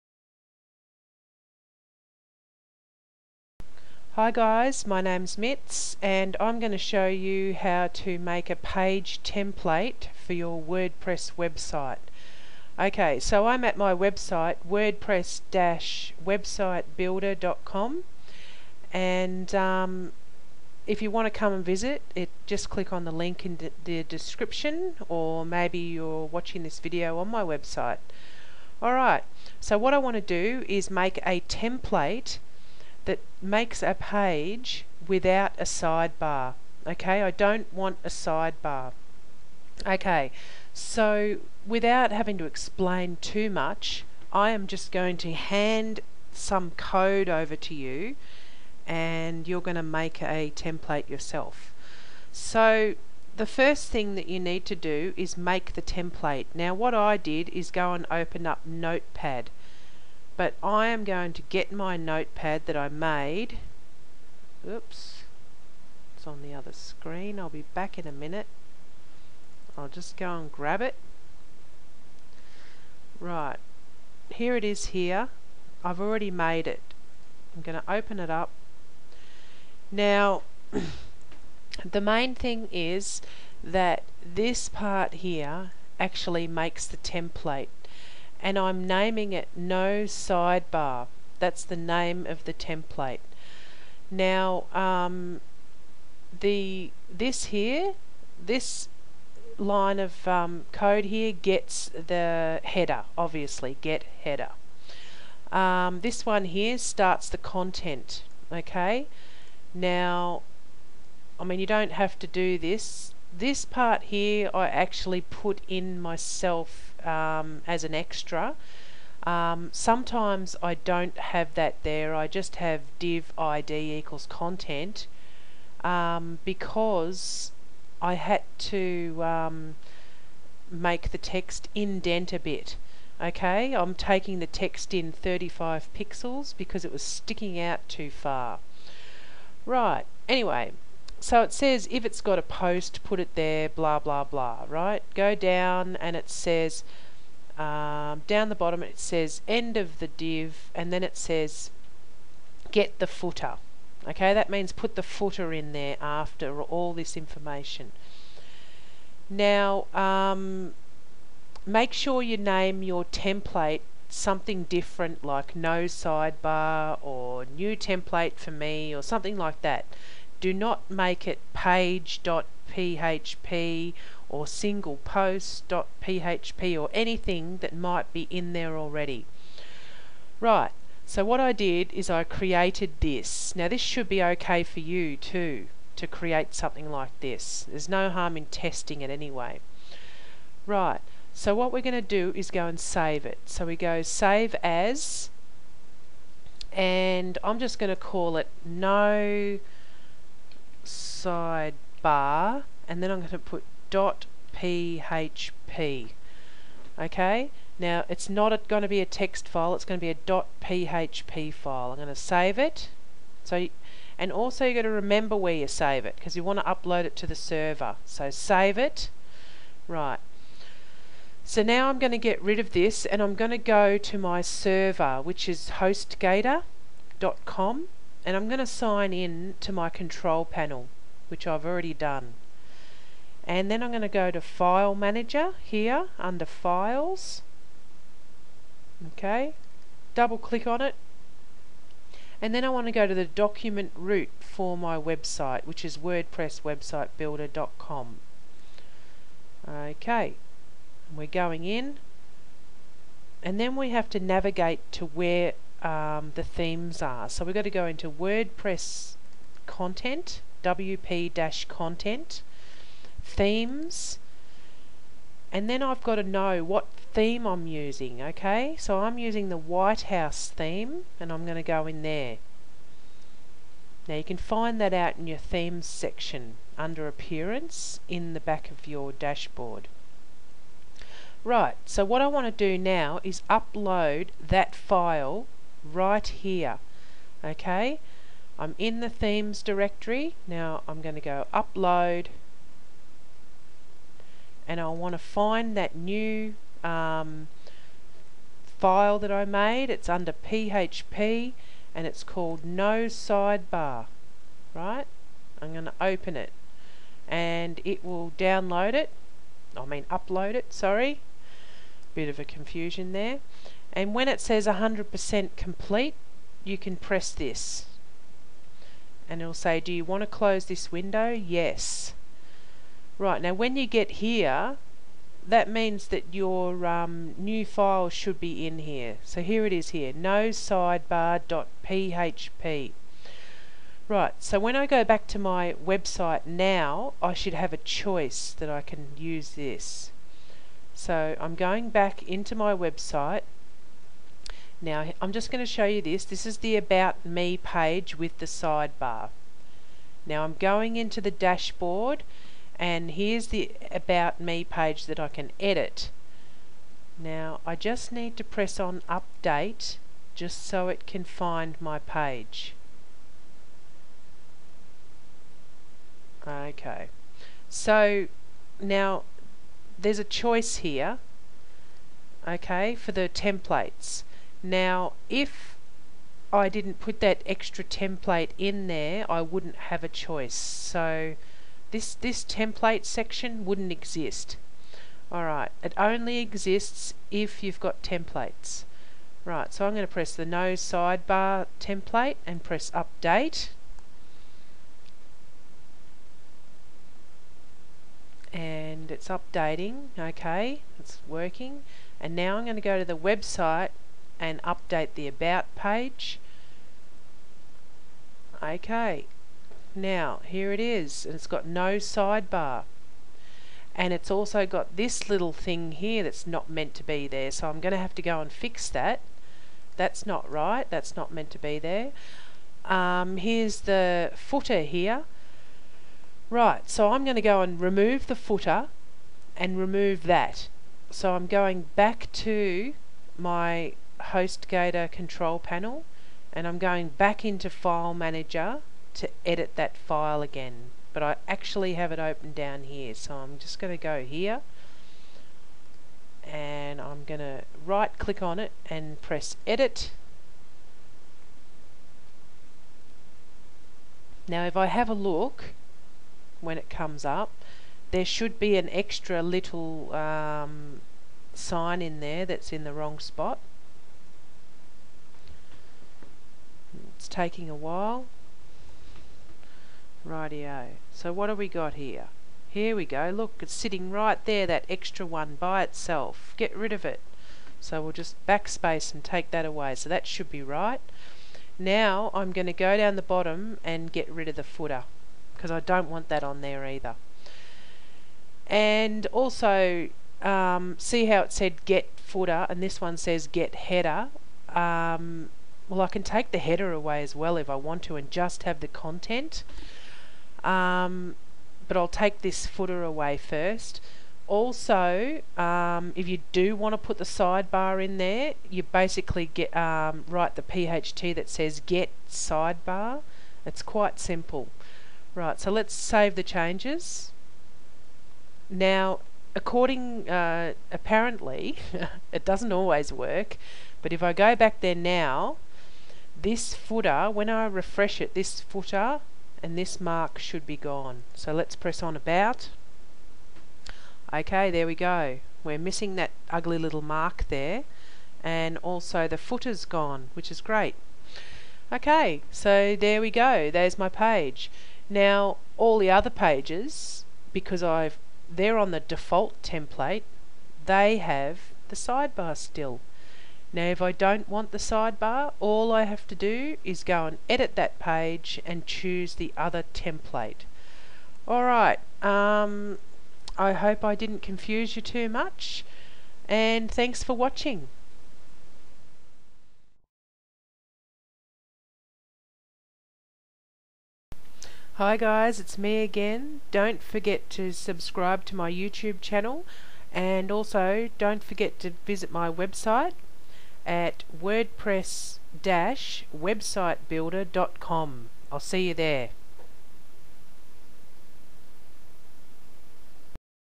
Make a Wordpress template page with no sidebar
Make a Wordpress template page with no sidebar. Create a new page template without a side bar or even a footer. Create different templates for your Wordpress Website.